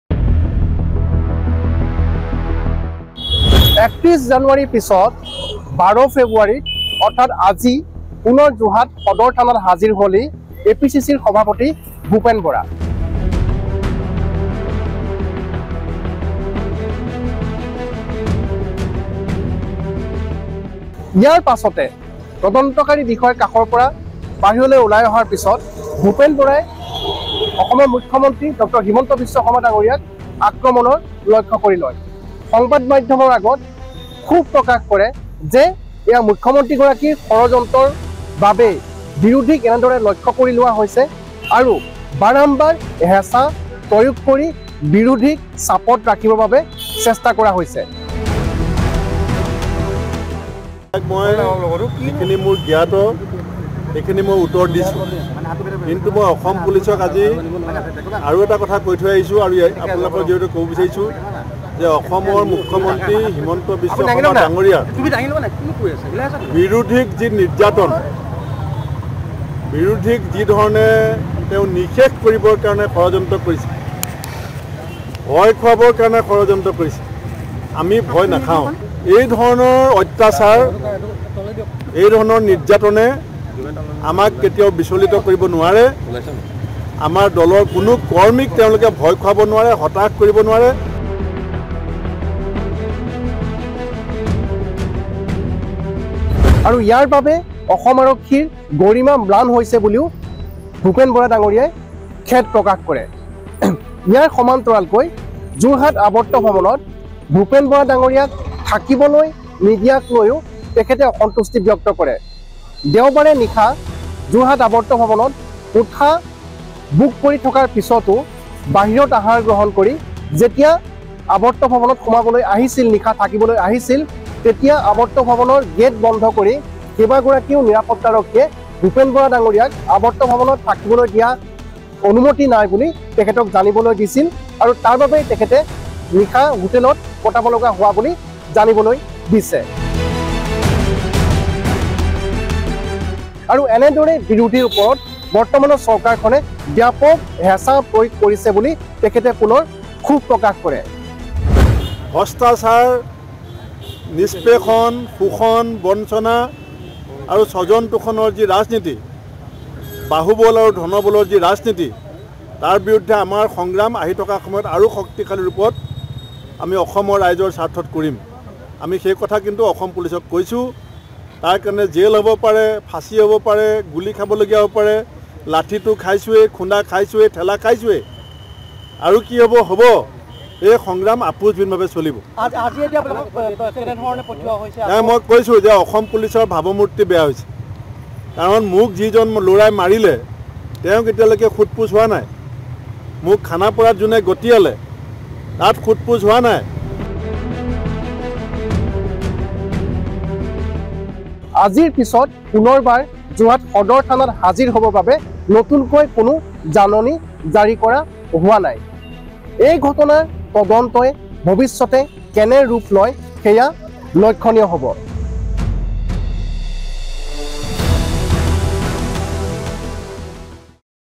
Bhupen Bora Latest News | ২য়বাৰৰ বাবে APCCৰ ৰাজ্যিক সভাপতি ভূপেন বৰাক জেৰা যোৰহাট আৰক্ষীৰ N18V
দ্বিতীয়বাৰৰ বাবে এপিচিচিৰ ৰাজ্যিক সভাপতি ভূপেন বৰাক জেৰা যোৰহাট আৰক্ষীৰ। জেৰাৰ পিছত গ্ৰেপ্তাৰ কৰিব বুলি চৰ্চা চলিছিল ভূপেন বৰাক গ্ৰেপ্তাৰ কৰাৰ পৰা বিৰত থাকিল যোৰহাট আৰক্ষী। ইফালে চৰকাৰৰ এয়া ষড়যন্ত্ৰ বুলি আখ্যা দিছে এপিচিচিৰ ৰাজ্যিক সভাপতিগৰাকীয়ে।